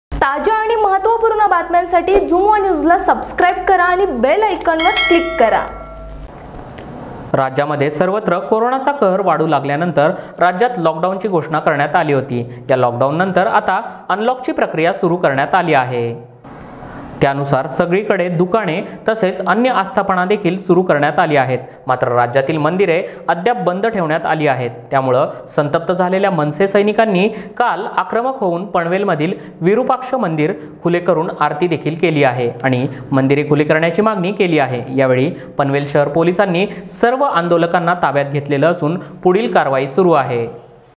पनवेल मध्ये मनसेकडून मंदिर उघडण्यासाठी आंदोलन
mandir andalan